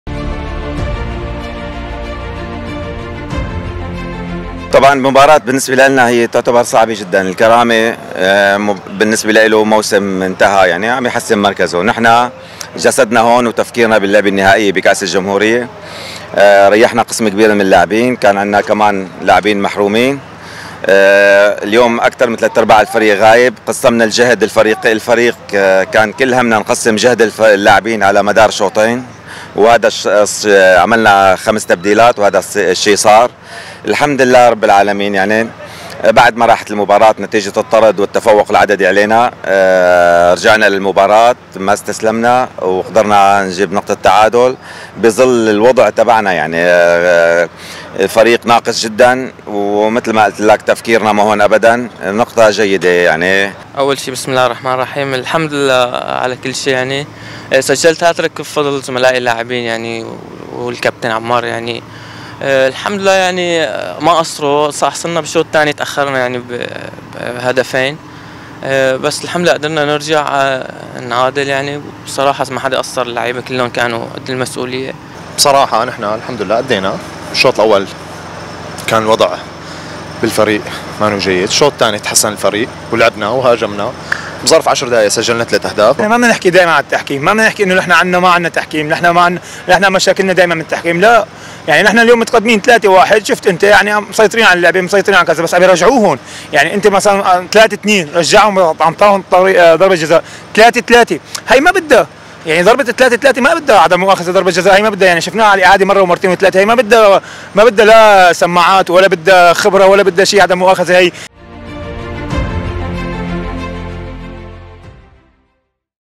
صدى الرياضة | أبرز تصريحات لقاء جبلة وضيفه الكرامة ضمن منافسات الجولة 25 للدوري السوري الممتاز
استطلاع : عبدالسلام أصفر
تصوير : احمد نعمة
مونتاج : كرم خواتمي